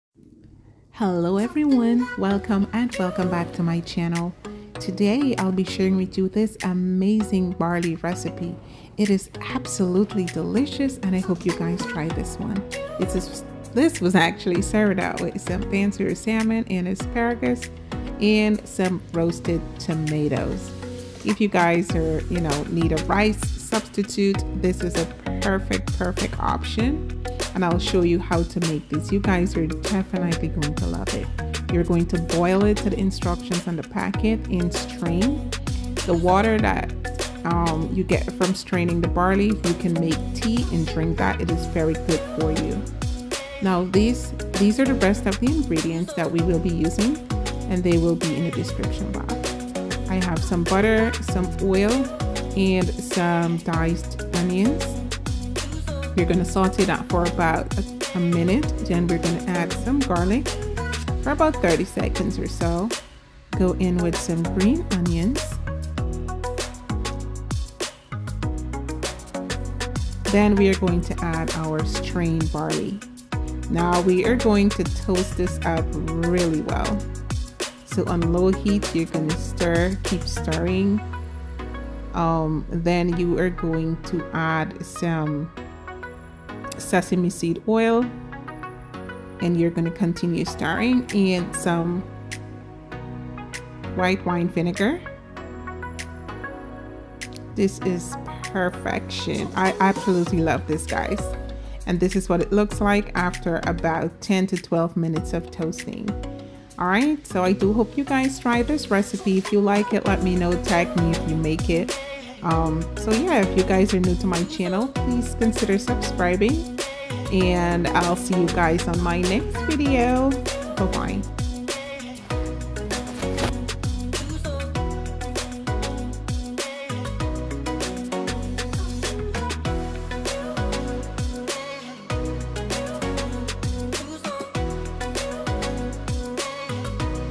Best Barely recipe/ irresistible recipe you must try
Ingredients
1 cup barely boiled and drain
1 tbsp butter
1 tbsp oil
1/4 onion
3 cloves diced garlic
Green onion
chili flakes
1 tbsp bouillon ( season to your preference)
1/4 tsp sesame oil
1 1/2 tsp white wine vinegar